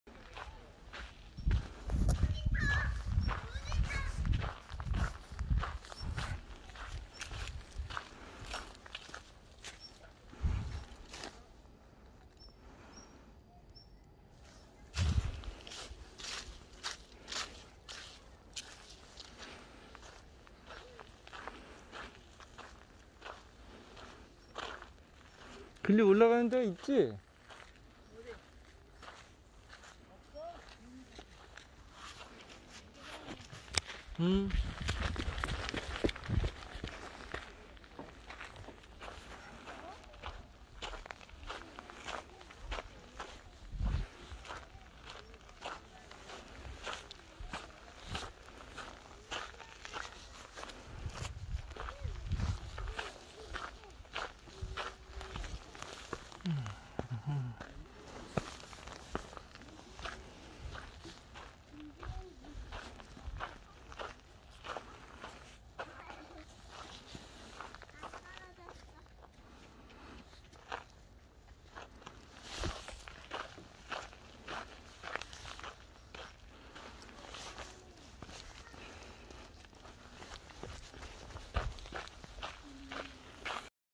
위례아이들과가볼만한곳-위례공원 유아숲
코로나시대

아가들과 나들이와서 운동하기딱좋은곳

추천합니다!